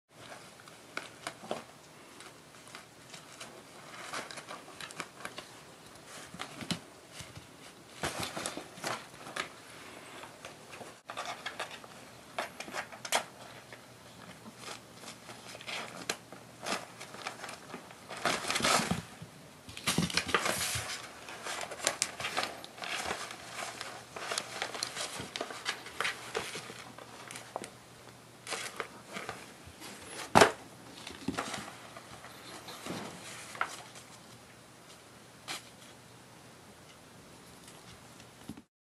子猫のいたずら３ - Kitten Mischief 3 -
宅急便ビジネスパックでいたずらをする子猫（おはぎ）です。